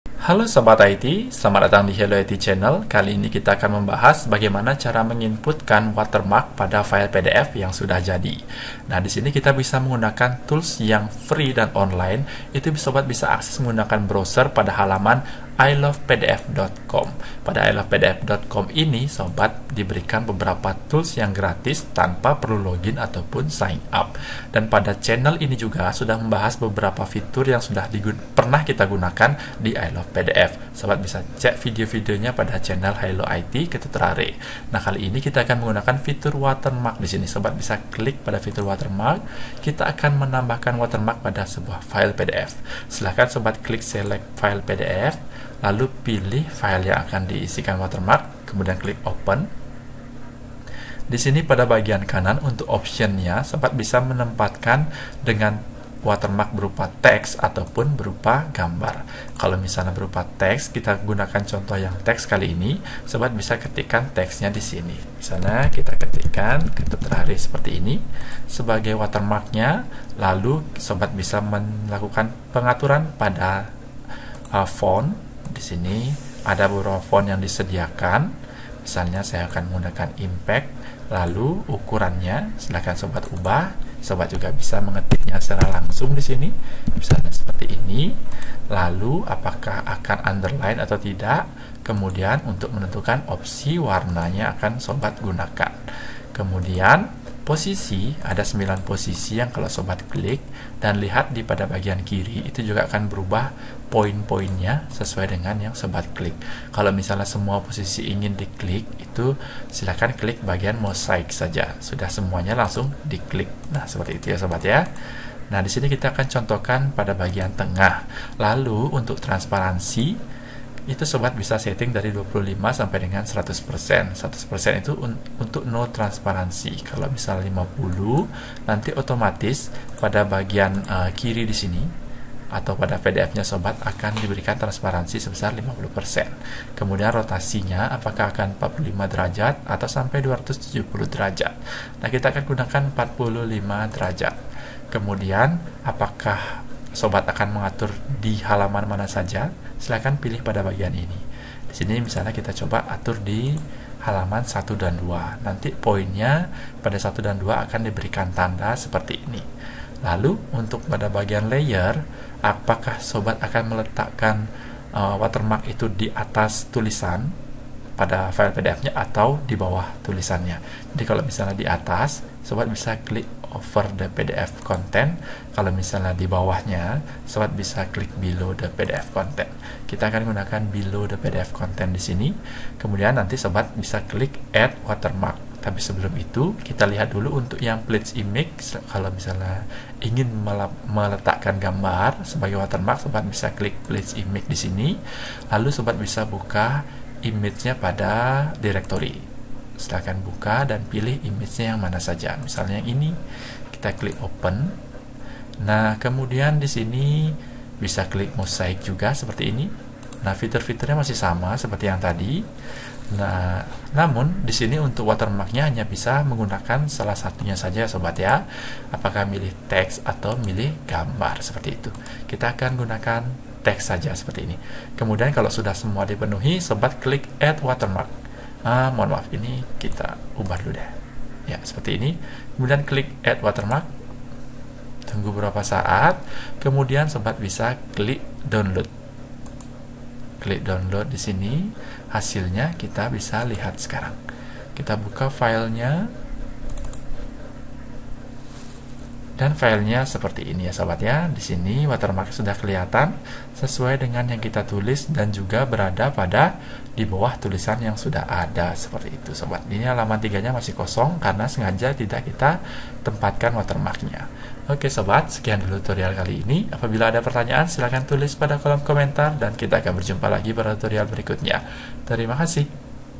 Cara Menambahkan Watermark Pada File PDF Yang Sudah Jadi PDF Lama
Halo sobat IT, mungkin dari sobat semua ada yang sudah memiliki PDF Lama dan ingin menambahkan watermark pada pdf tersebut. Tujuannya mungkin untuk di upload dan melindungi hasil karya sobat sendiri agar tidak disebarluaskan untuk kepentingan yang salah.

Nah berikut ini kita akan bahas salah satu solusi menambahkan watermark pada PDF lama dengan mudah , cepat , dan gratis tentunya. Sobat bisa menggunakan salah satu fitur di I Love PDF ini.

Sobat bisa menambahkan watermark berupa tulisan ataupun berupa gambar. Pastinya sobat yang penasaran, silahkan klik videonya dan selamat menonton. Terimakasih